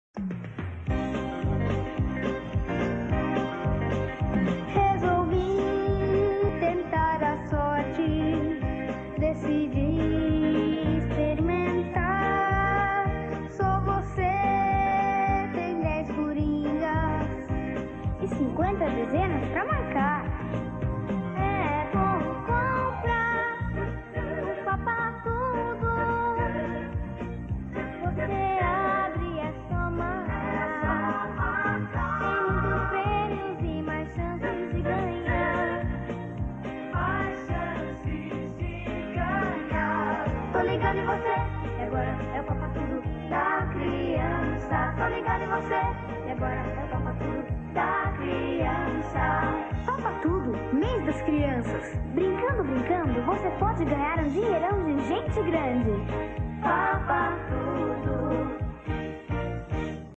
Comercial do titulo de capitalização 'Papa tudo com Sandy e Junior'. 29/09/1994.
NÃO COPIE ESTE VÍDEO EM SEU CANAL.